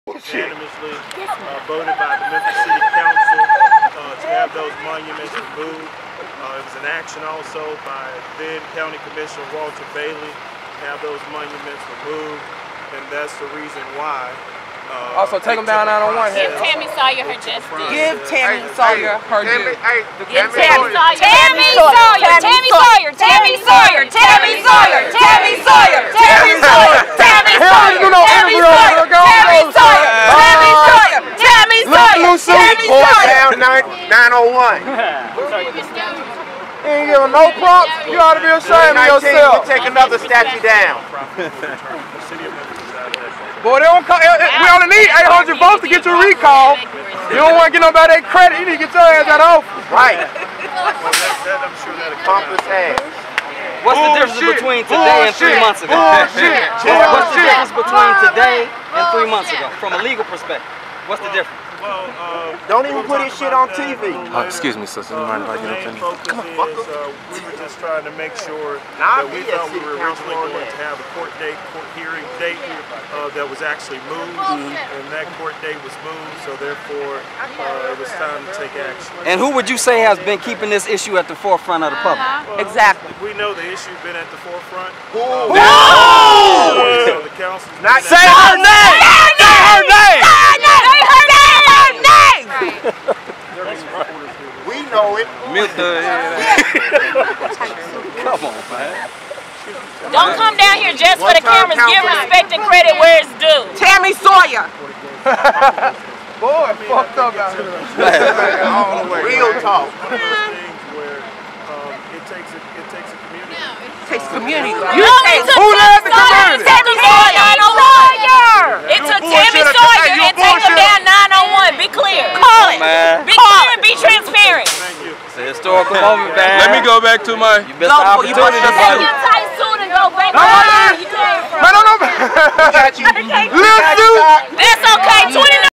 TSDtv: TAKEN DOWN: What happened when Berlin Boyd failed to mention Tami Sawyer and TakeEmDown901
TSD Freelance photojournalist Johnathan Martin provides raw video of the removal of the Nathan Bedford Forrest statue. (NOTE: Clip may include explicit language).